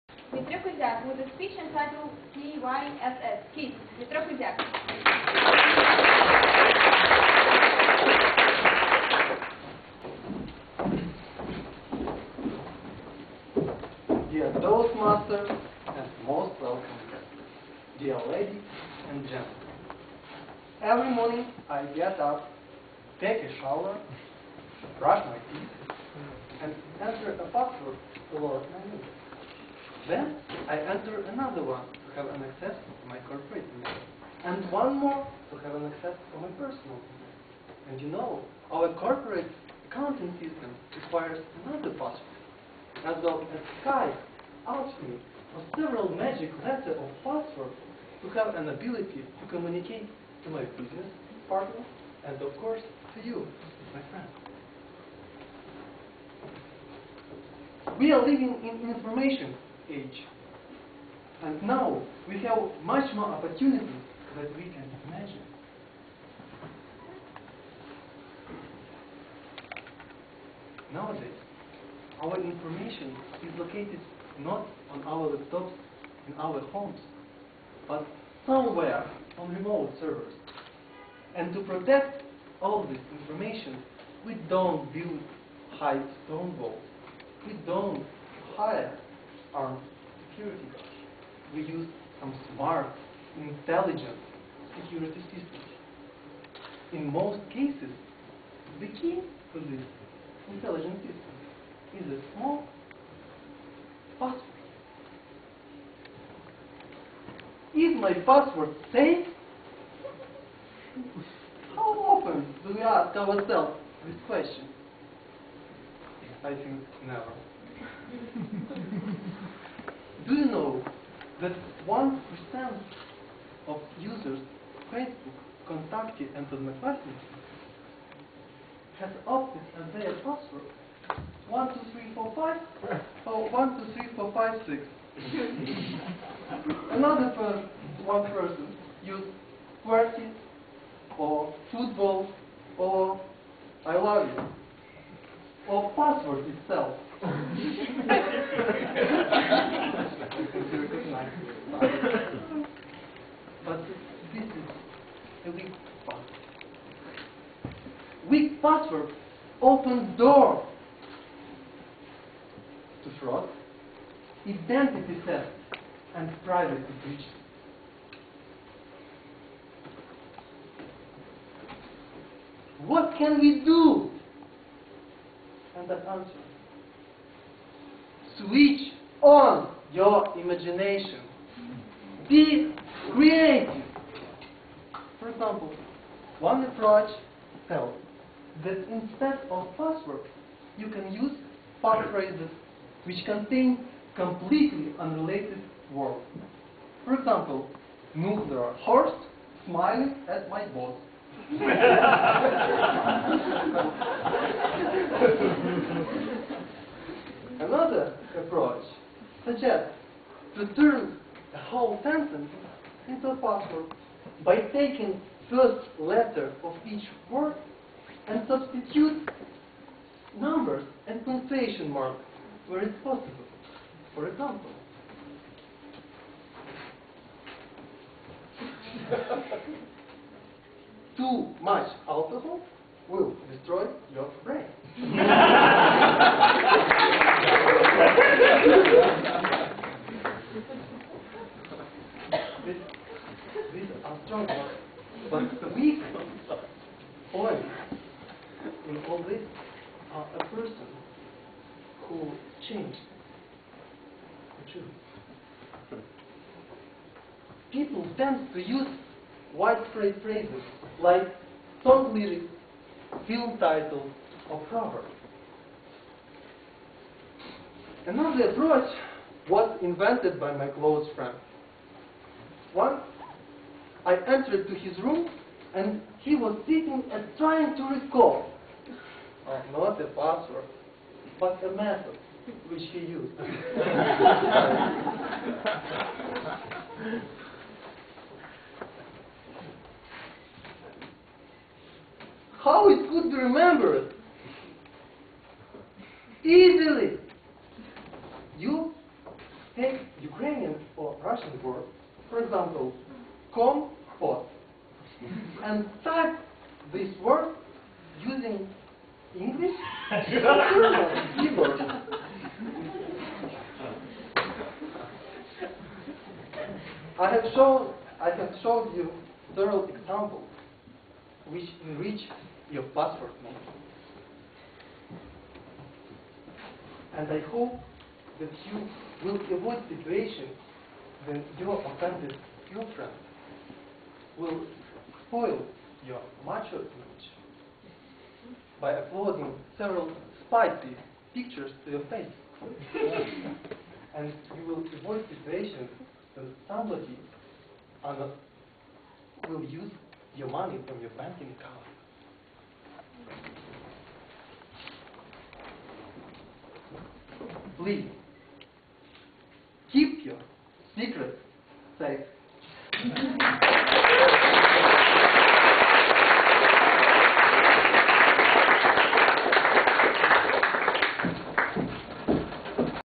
Английский в Киеве (Украина): Toastmaster Dmytro: What is KY$$ ?
ВЫСТУПЛЕНИЕ НА 8-м ВСЕУКРАИНСКОМ КОНКУРСЕ ОРАТОРСКОГО ИСКУССТВА КЛУБОВ АНГЛИЙСКОГО ЯЗЫКА ТОАСТМАСТЕРОВ (АПРЕЛЬ 2012).
Toastmaster Dmytro: KISS or KY$$.

Speech at 8th All-Ukrainian Toastmasters Speech Contest-2012. TIME: April 8th, 2012.
Toastmasters are English-Speaking Clubs where members develop public speaking skills in English. :)
Recorded by Entomologist in Ukraine.